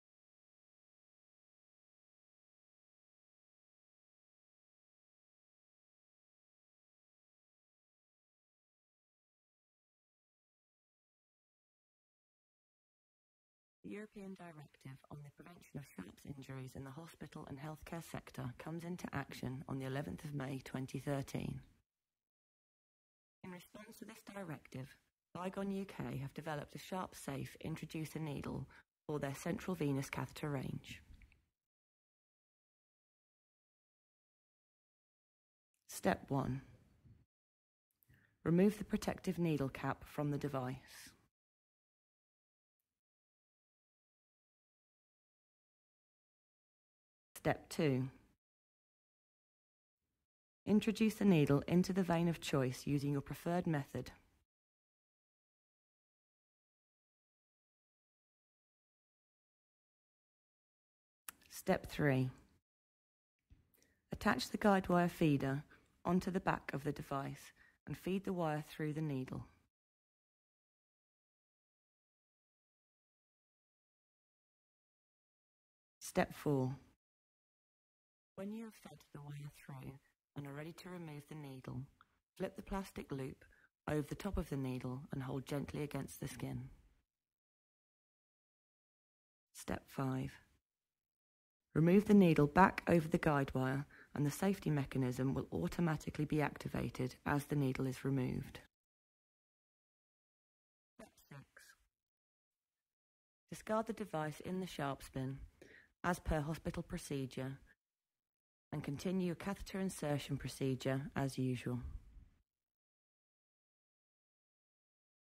SeldiSafe® - Helping to reduce the risk of sharps injury during CVC insertion
With 40,000 incidents reported each year in the UK (and at least as many go unreported), this is a major cause for concern for the NHS.

SeldiSafe is an 18G 70mm introducer needle which can be used instead of your conventional introducer needle for CVC insertion.

SeldiSafe has been developed in response to the EU Directive on the prevention of sharps injuries in the hospital and healthcare sector, which comes into action on 13th May 2013.